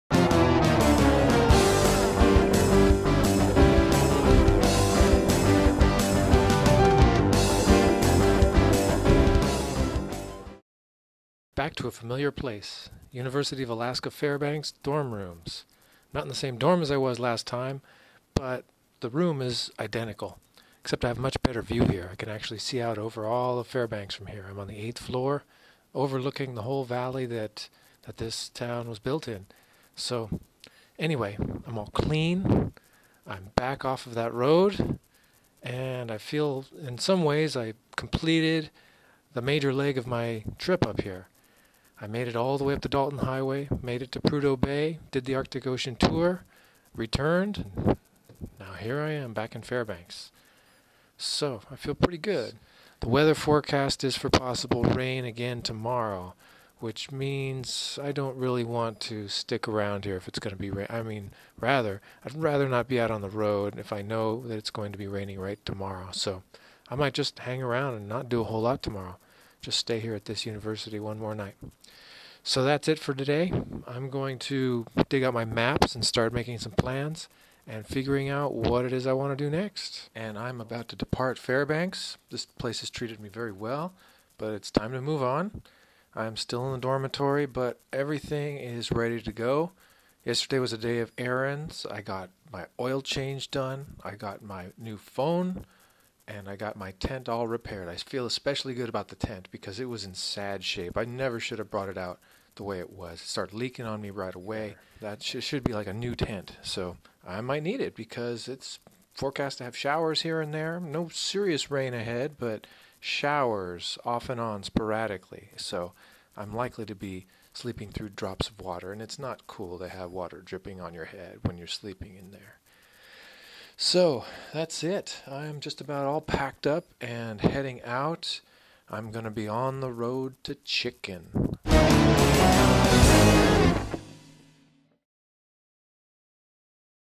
Arctic Motorcycle Camping: Days 11 & 12 Ride Reports
With the tour of Prudhoe Bay and the Arctic Ocean complete, it's back to Fairbanks to prepare for the next leg of arctic motorcycle camping.